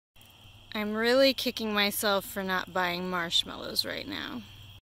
The Carriage House 9: Marshmallow Dreaming!
Wishing i had some marshmallows to toast while burning all these downed tree branches.